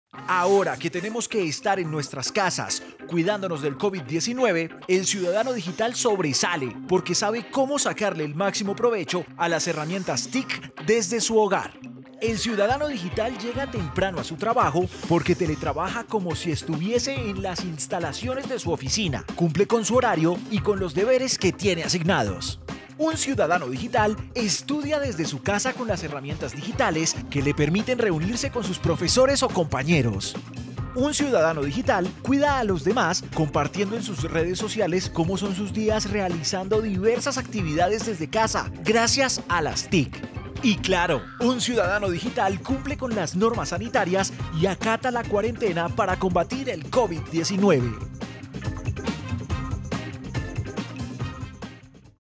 En casa usa las TIC
Saber usar las herramientas digitales y acatar las indicaciones sanitarias de las autoridades hace a los colombianos un CiudadanoDigital. En estos tiempos las TIC se convierten en el mayor aliado para continuar con muchas de nuestras actividades laborales.